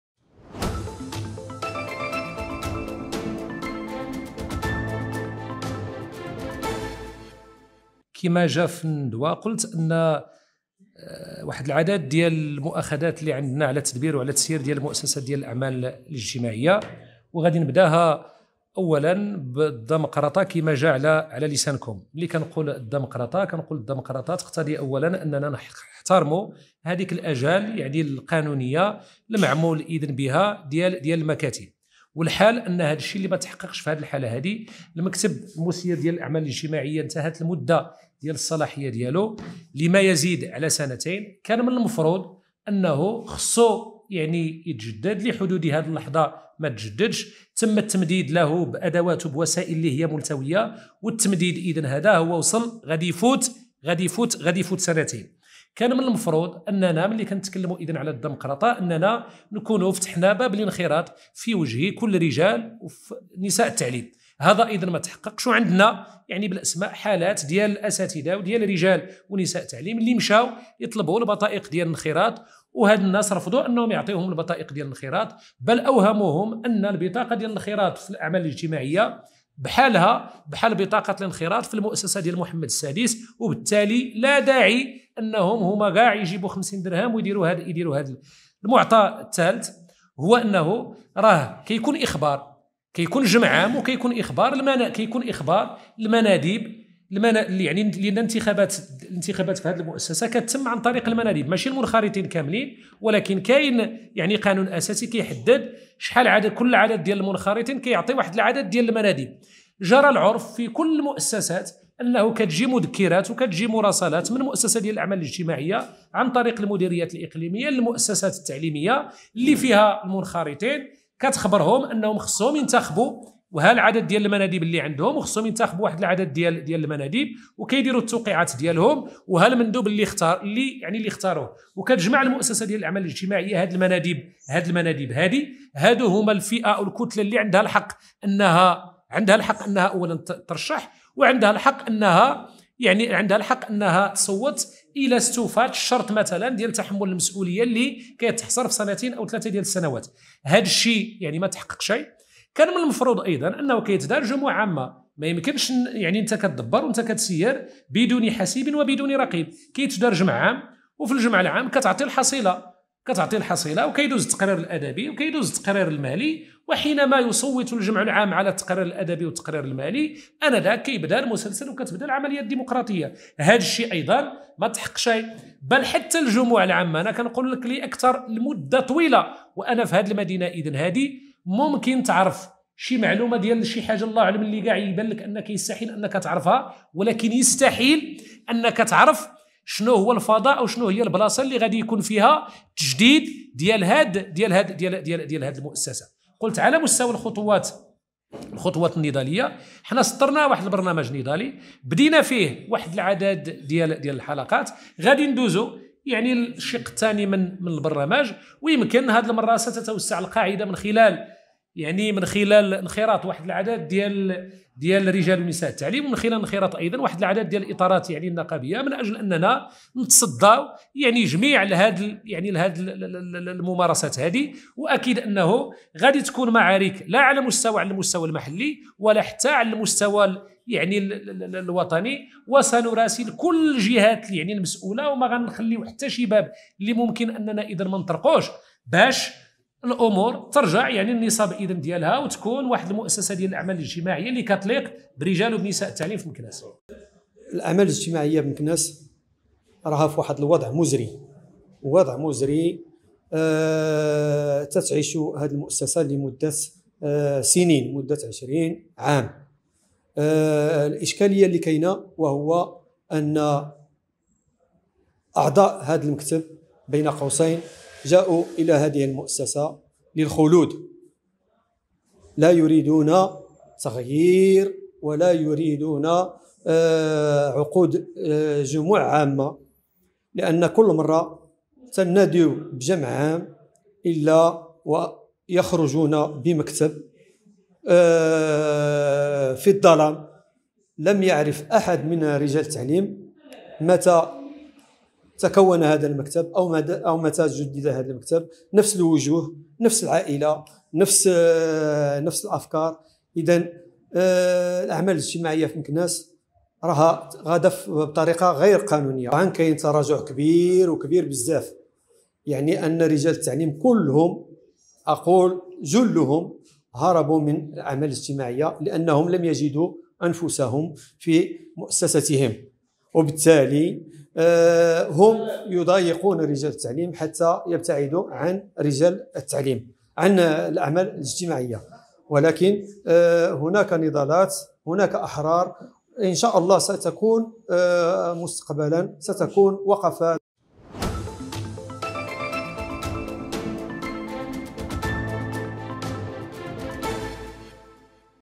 نقابيون يستنكرون " خلود " مسؤولين في مؤسسة الأعمال الاجتماعية بمكناس في مناصبهم
يتطرف الفيديو الى اشكالية دمقرطة مؤسسة الأعمال الاجتماعية بمكناس حيث انتهت صلاحية المكتب الحالي؛ كما ينتقد احتكار المناصب من طرف نفس الأشخاص لفترة طويلة؛ داعيا الوزارة الوصية الى التدخل لاعادة الأمور  الى نصابها

# أخبار_عاجلة